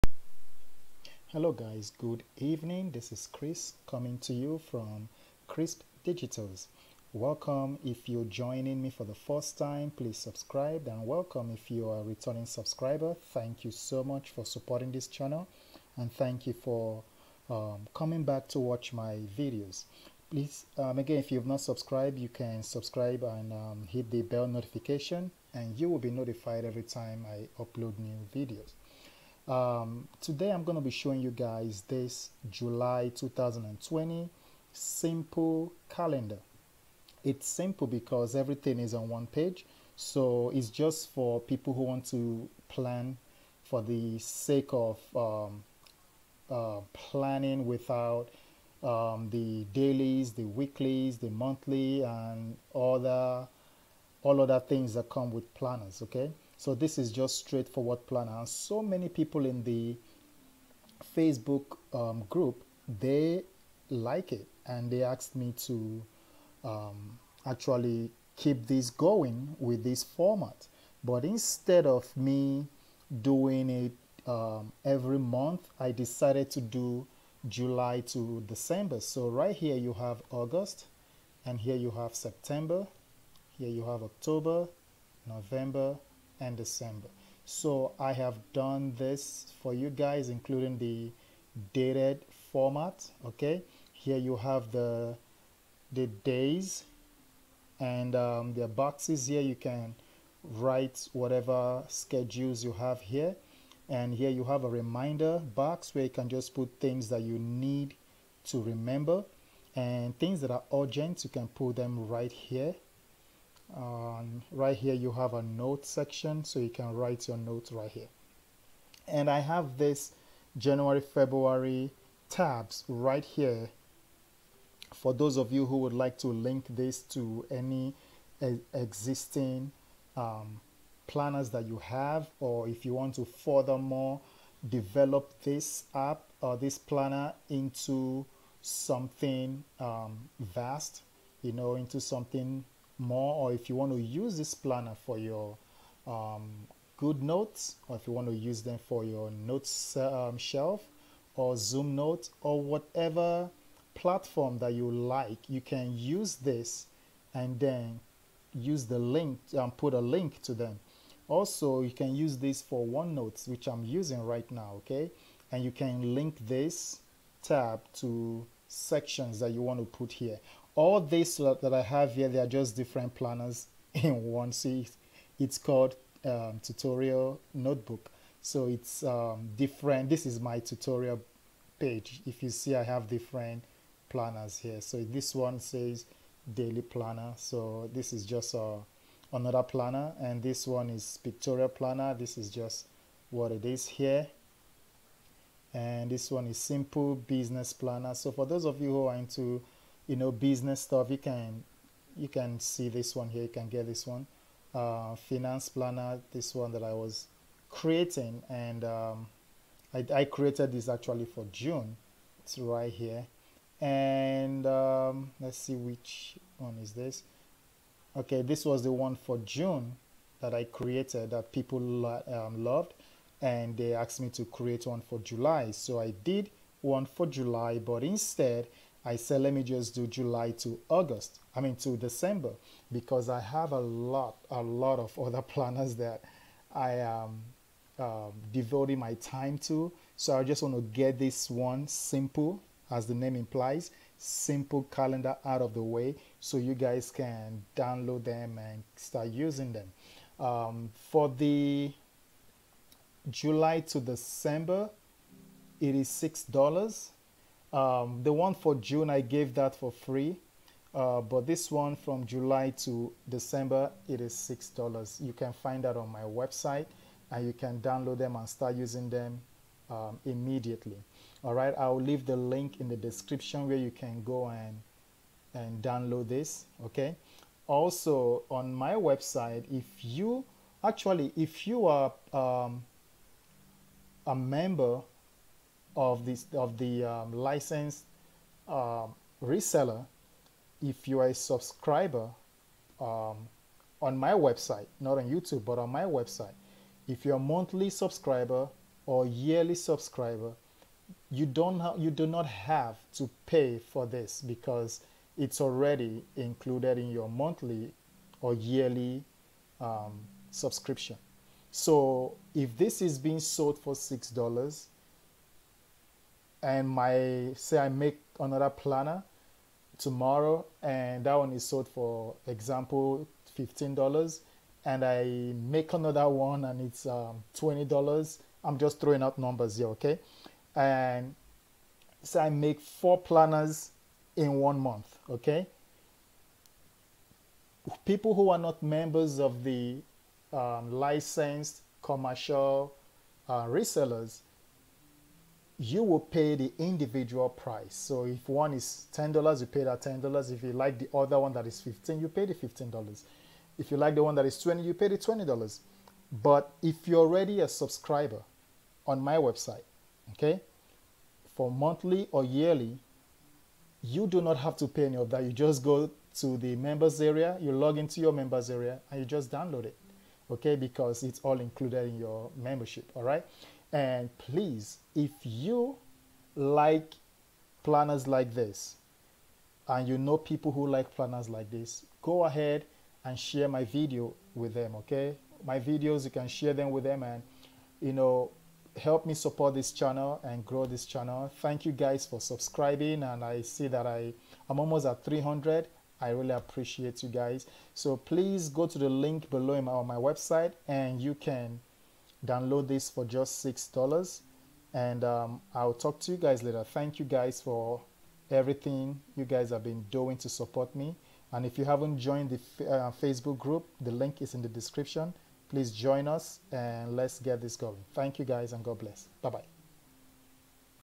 Simple Calendar For July Through December
Hello guys! Today I will be showing you guys the Simple Calendar that I created. It was created for those who just want a straightforward calendar without the need of other aspects of a regular planner.  This is simple to use and can be further customized to fit your needs.